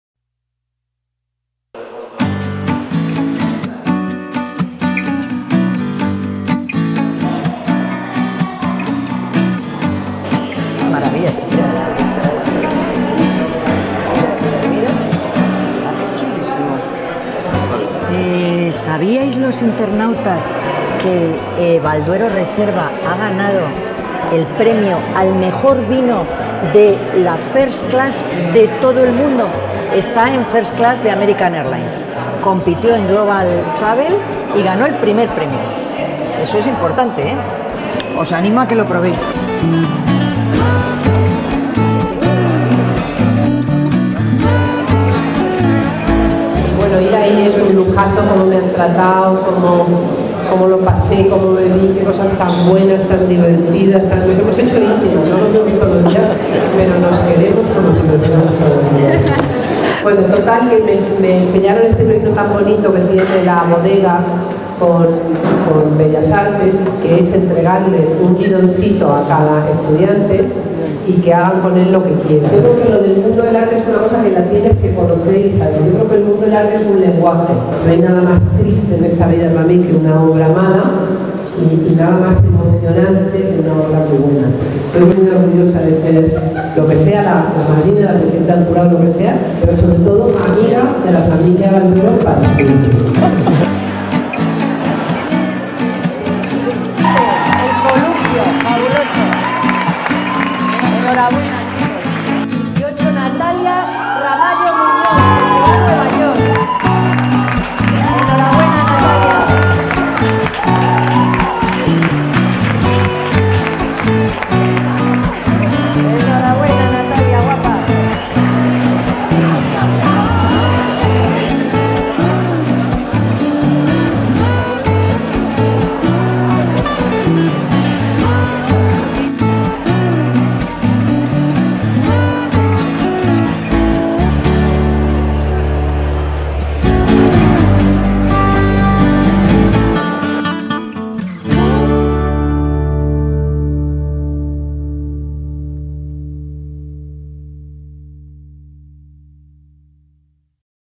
Valduero y las Bellas Artes 2018
Valduero ha celebrado junto a Agatha Ruiz de la Prada e IV Certamen de Valduero con las Bellas Artes.

Bodegas Valduero, Vinos, Wines ,D.O. Ribera del Duero.